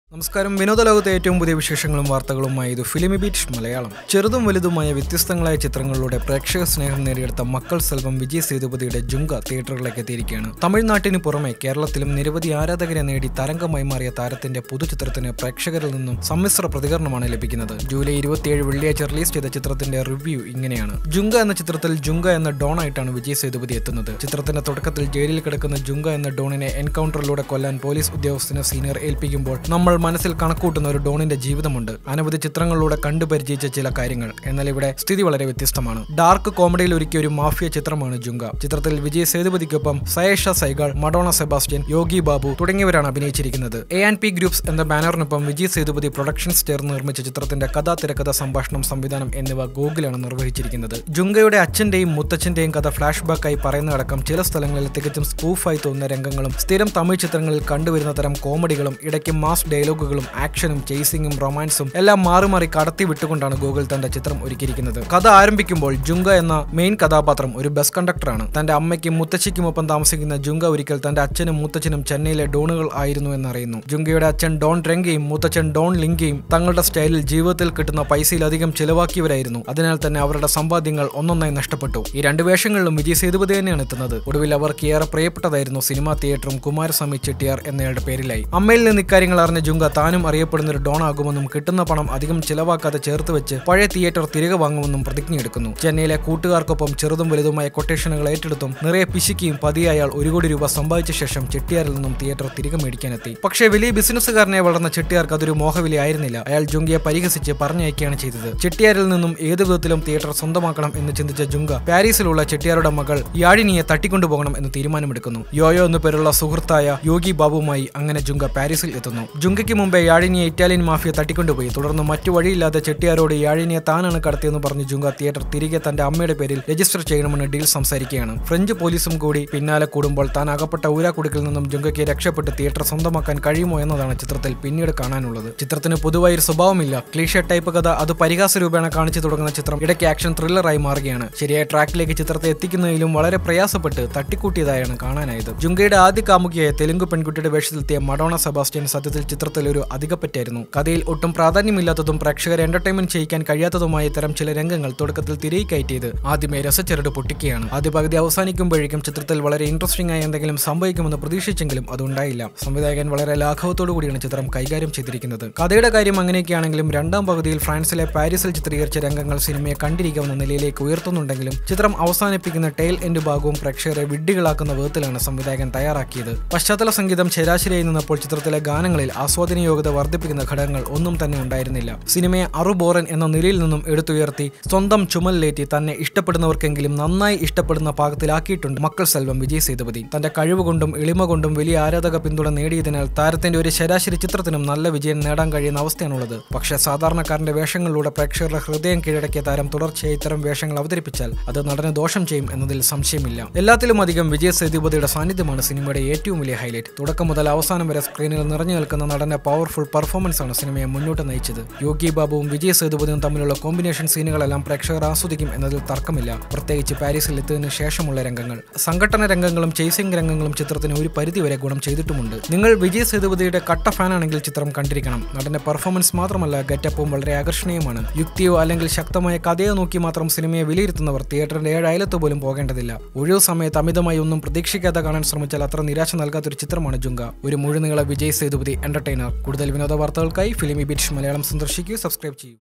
Junga Movie Review | വിജയ് സേതുപതി ചിത്രം “ജുങ്ക” | Vijay Sethupathi | filmibeat Malayalam
Junga movie review
ചെറുതും വലുതുമായ വ്യത്യസ്തങ്ങളായ ചിത്രങ്ങളിലൂടെ പ്രേക്ഷക സ്നേഹം നേടിയെടുത്ത 'മക്കൾ സെൽവം’ വിജയ് സേതുപതിയുടെ 'ജുങ്ക’ തീയറ്ററുകളിലേക്ക് എത്തിയിരിക്കുകയാണ്. തമിഴ്നാട്ടിന് പുറമെ കേരളത്തിലും നിരവധി ആരാധകരെ നേടി തരംഗമായി മാറിയ താരത്തിന്റെ പുതുചിത്രത്തിന് പ്രേക്ഷകരിൽ നിന്നും സമ്മിശ്ര പ്രതികരണമാണ് ലഭിക്കുന്നത്. ജൂലൈ 27 വെള്ളിയാഴ്ച്ച റിലീസ് ചെയ്ത ചിത്രത്തിന്റെ റിവ്യൂയിലേക്ക്.